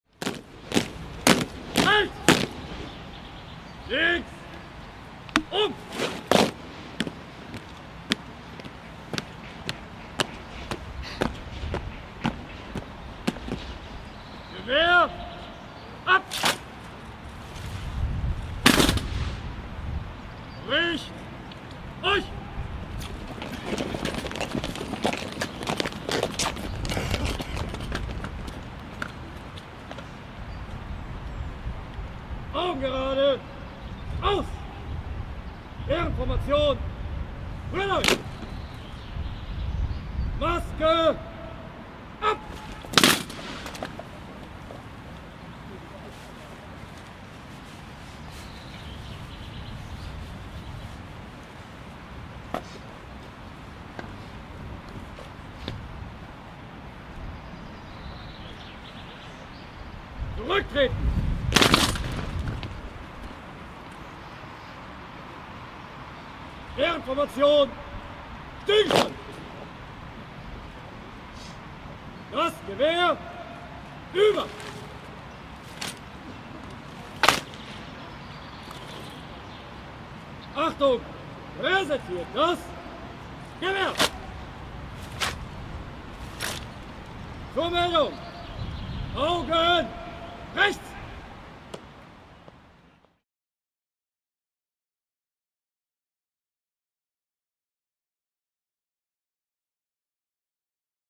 Maske ab (4)
Kommando "Maske ab" zum Beginn des Appells anlässlich der Übergabe eines Fahnenbandes des Regierenden Bürgermeisters von Berlin an das Wachbataillon und des Verdienstordens des Landes Berlin an Brigadegeneral Henne in der Julius-Leber-Kaserne. Die Ehrenformation wird durch die 4. Kompanie des Wachbataillons (Marineinfanterie) gestellt. Führer der Ehrenformation ist Major Timme, Chef der 1. Kompanie. Führer der Paradeaufstellung ist Oberstleutnant Bienas, Stellvertreter des stellvertretenden Kommandeurs des Wachbataillons, Oberstleutnant Homann. Der Appell wurde durch das Heeresmusikkorps Neubrandenburg unter Leitung von Oberstleutnant Prchal begleitet.